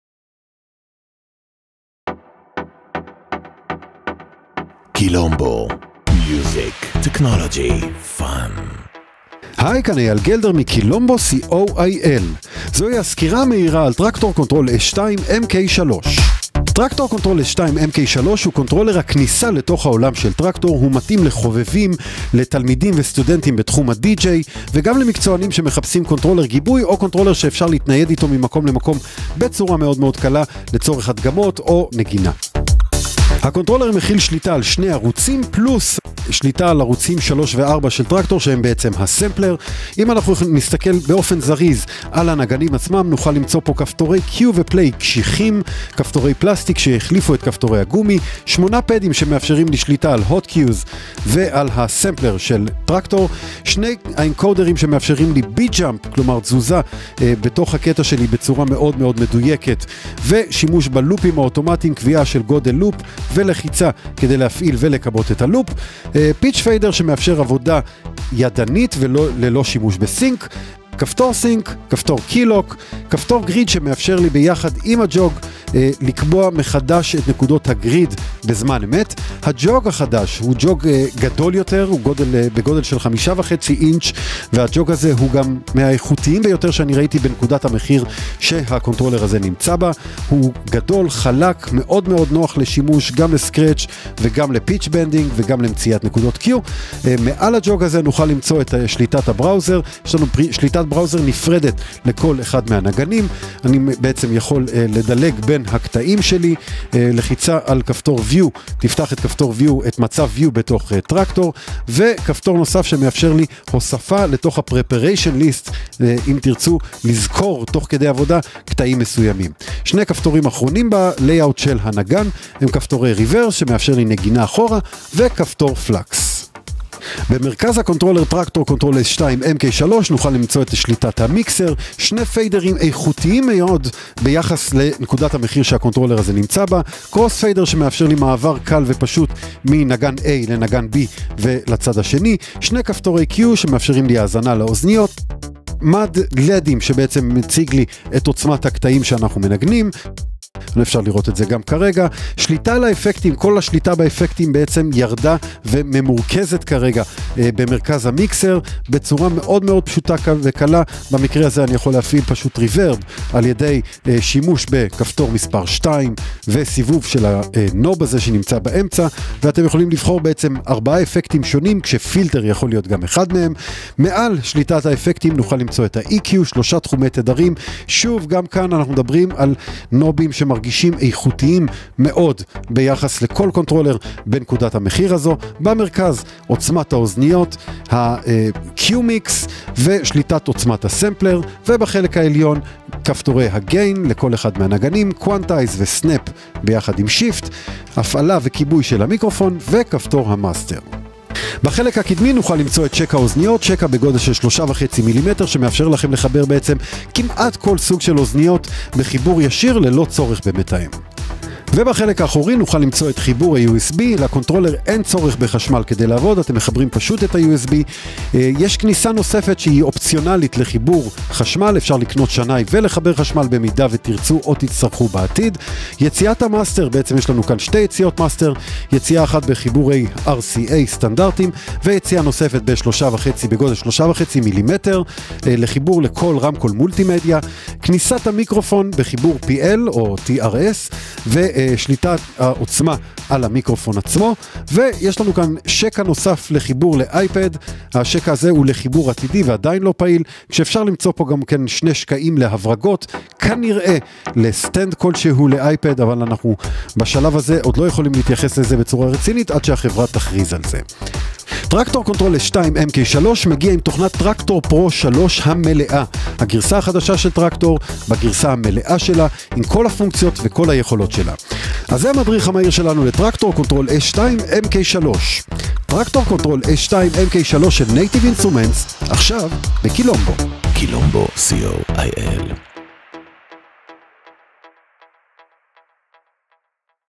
קונטרולר קומפקטי לדיג'יי - Traktor Kontrol S2 Mk3
סקירה על הקונטרולר החדש של Native Instruments, הקונטרולר הקומפקטי Traktor Kontrol S2 Mk3.